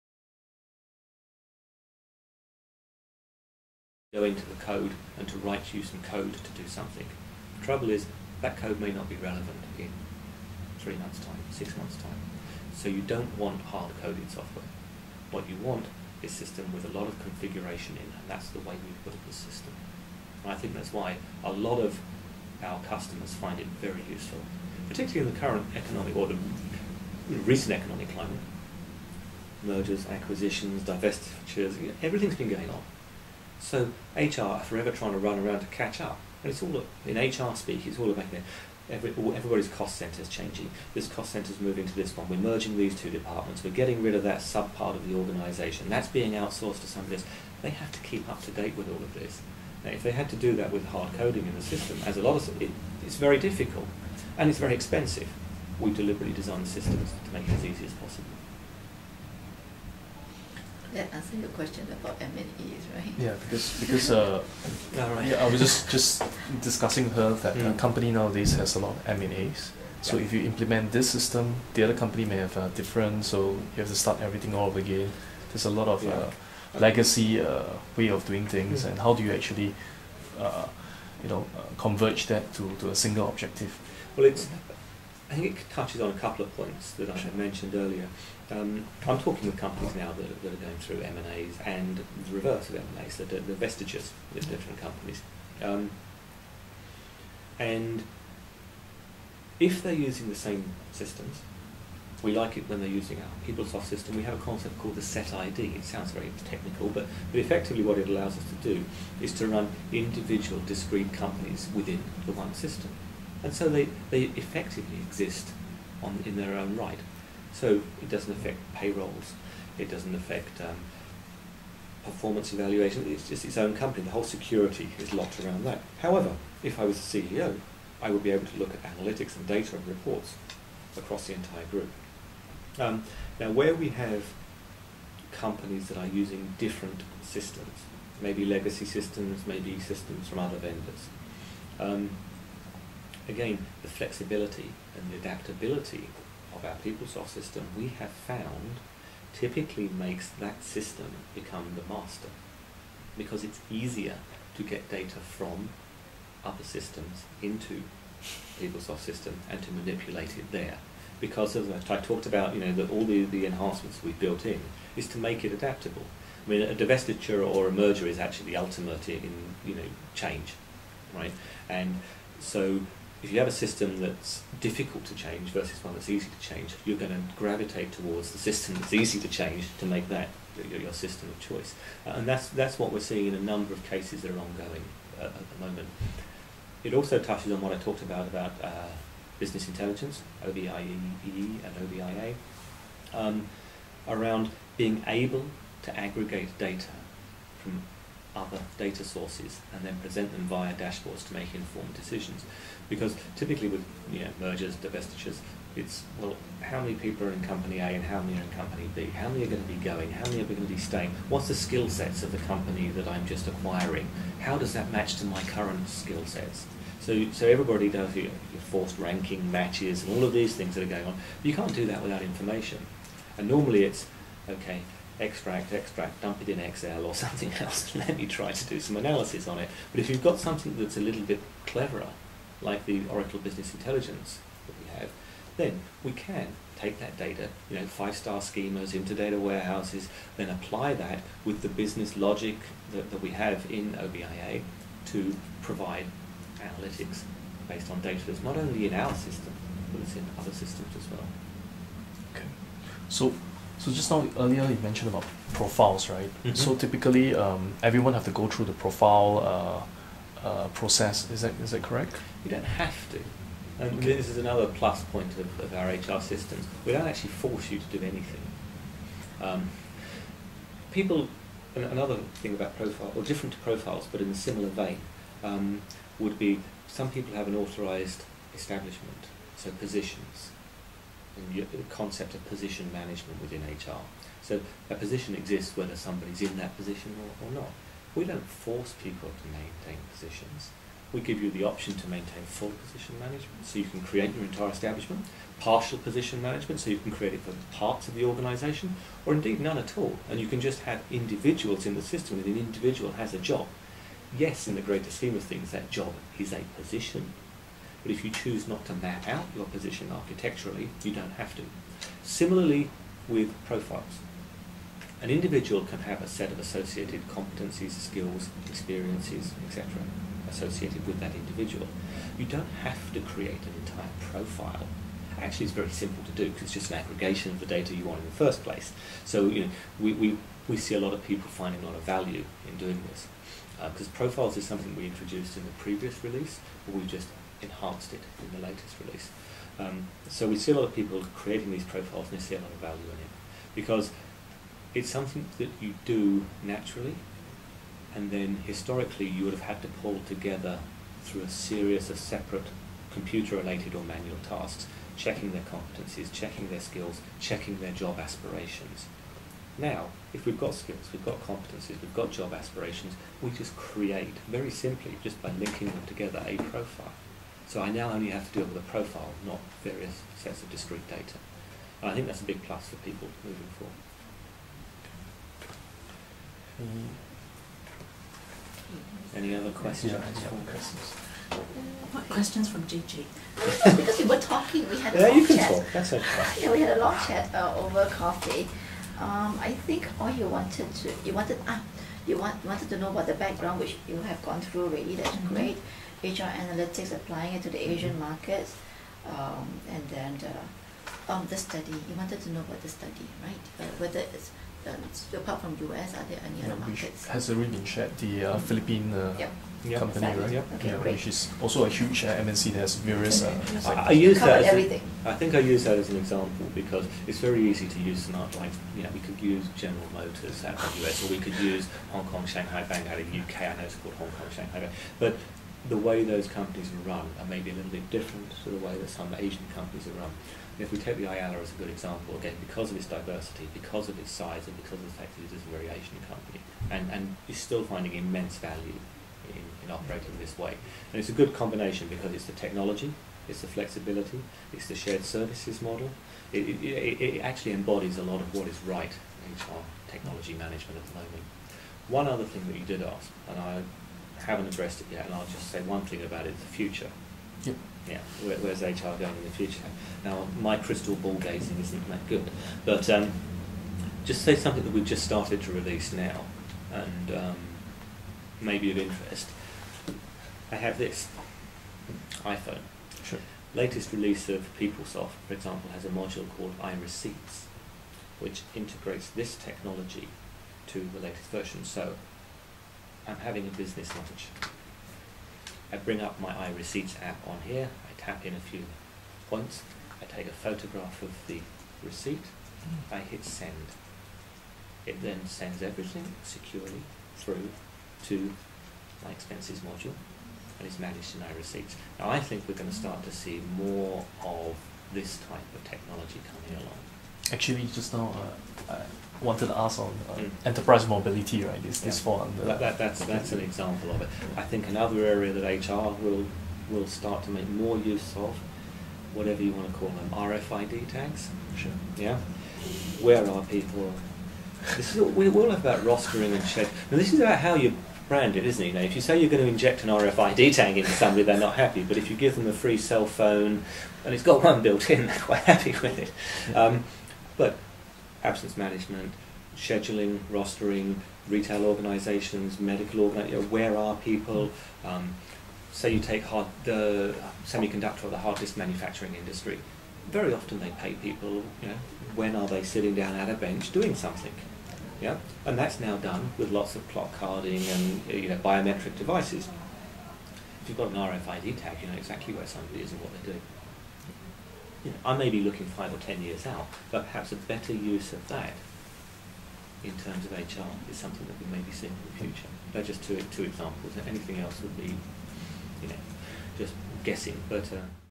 Tim Darton shares on Oracle Peoplesoft HCM 9.1
Oracle has recently released PeopleSoft Enterprise Human Capital Management (HCM) 9.1 & PeopleSoft Enterprise PeopleTools 8.50. Tim Darton, Senior Director (Oracle Apps Sales Consulting, APAC) shares on his views on the future of HR solutions & gives a brief overview of HCM 9.1& Oracle's HR Analytics.